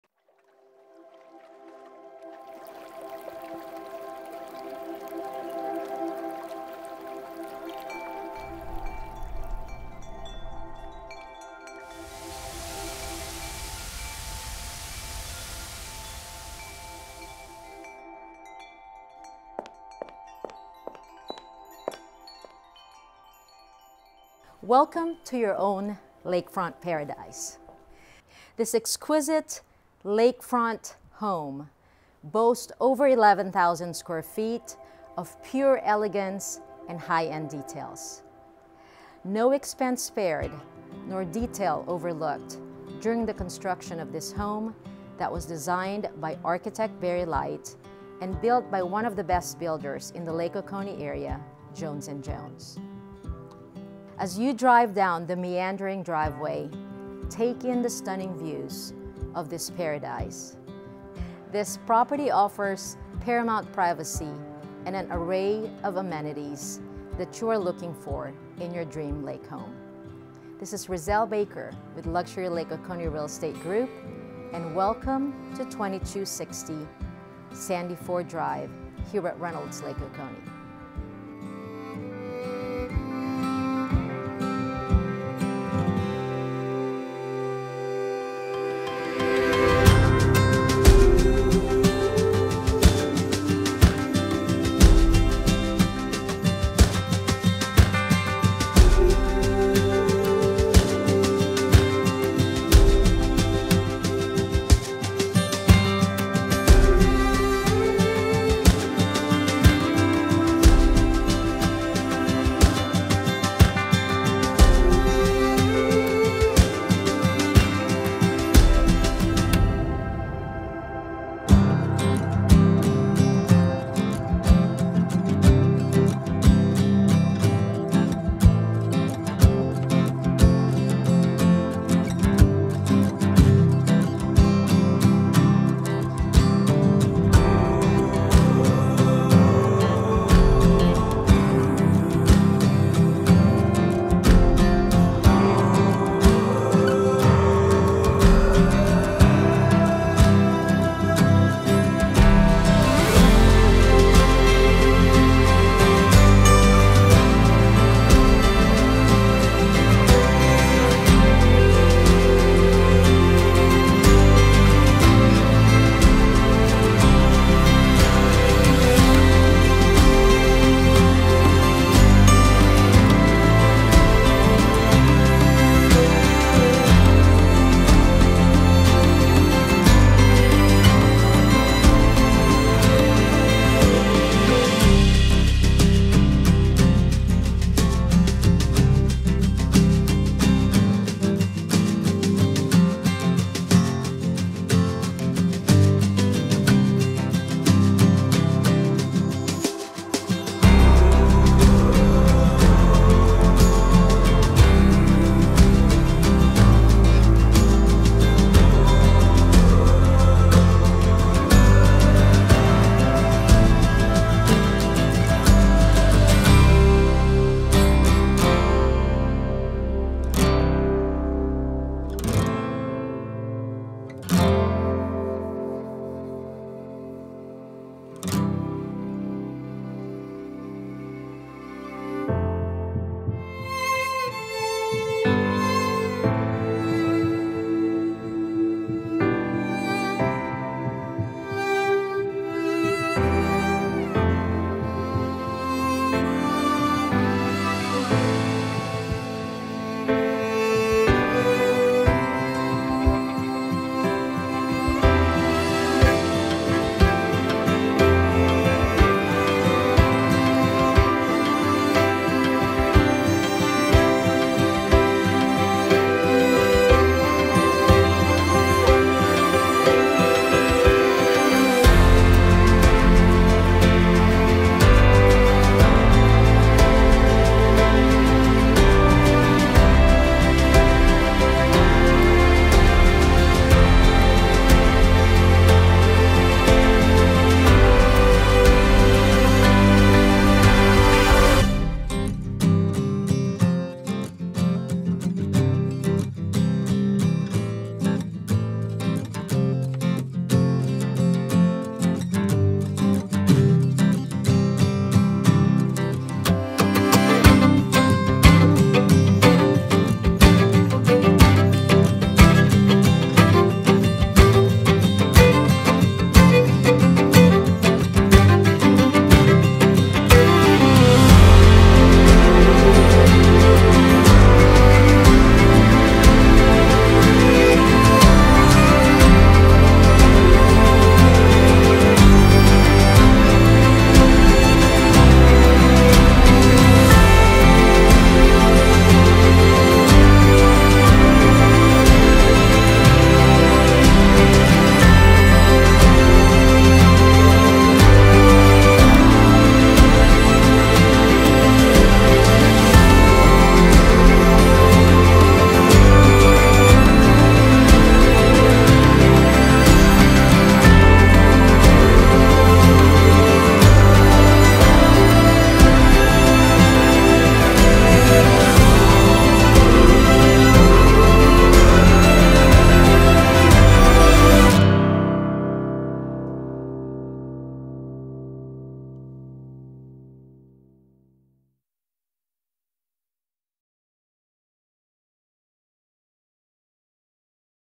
Riezl Baker Lake Oconee Luxury Realtor presents 2260 Sandy Ford NEW LAKEFRONT STUNNER CUSTOM HOME
Riezl Baker, Lake Oconee's Luxury Realtor Luxury Lake Oconee Real Estate presents another fantastic Lake Oconee listing with all the belles and whistles